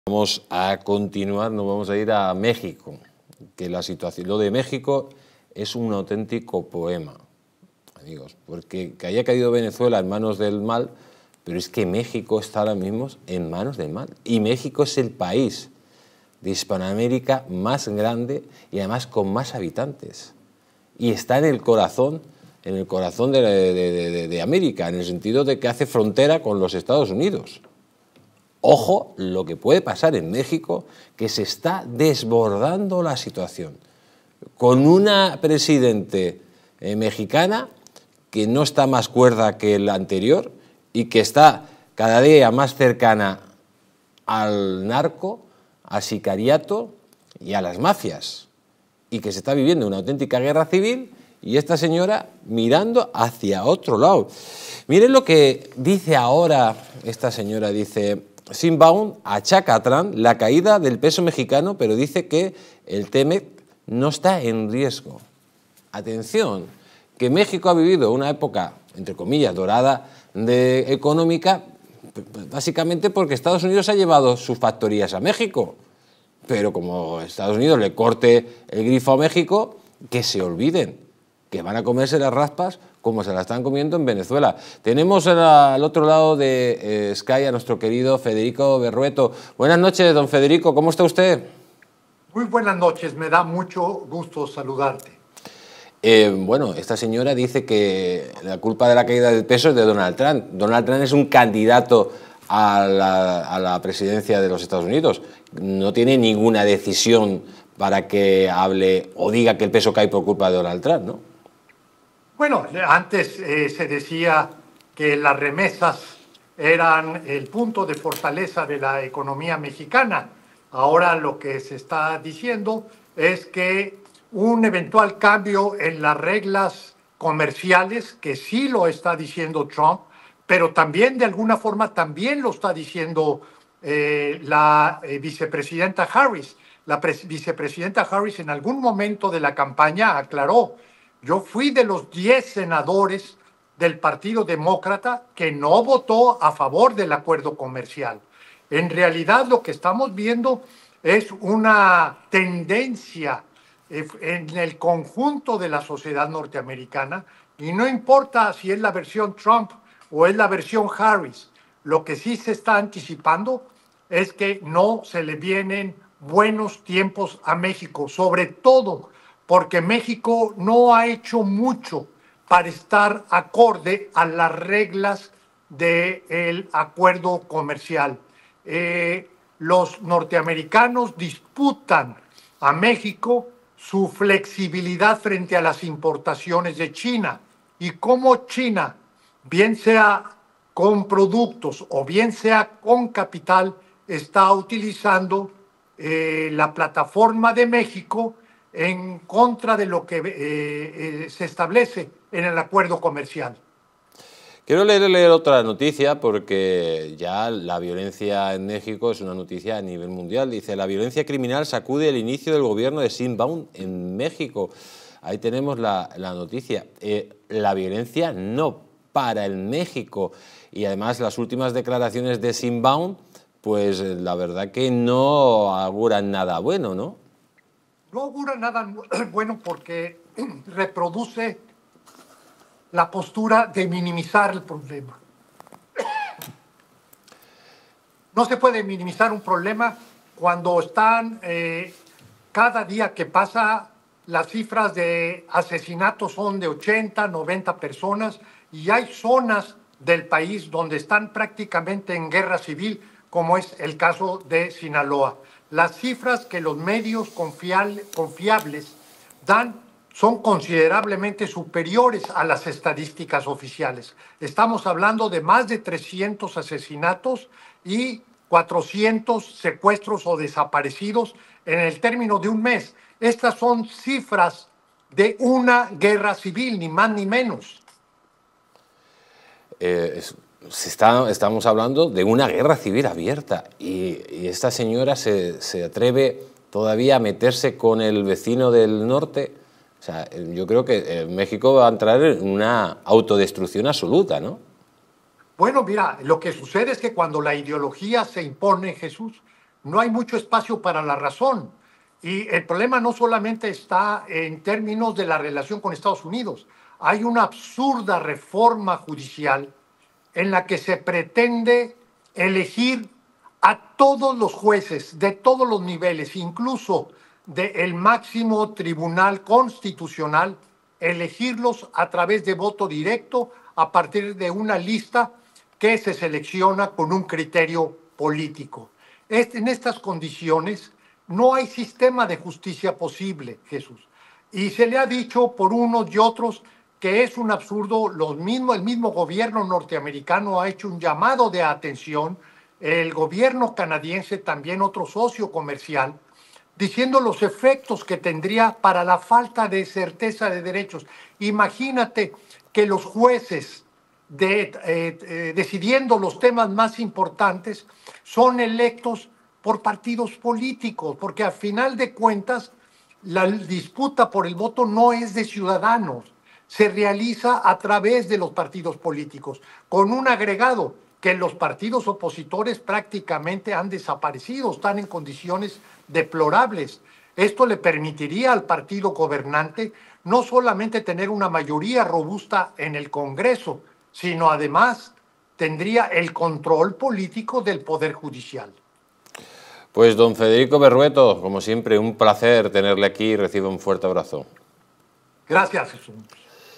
🔴MÉXICO🔴Sheinbaum declara la guerra aTrump y le culpa de la caída del peso mexicano
En este video, Sheinbaum declara la guerra a Trump y le culpa de la caída del peso mexicano. ¡Mira esta noticia en DistritoTV ahora! ¡No te lo pierdas!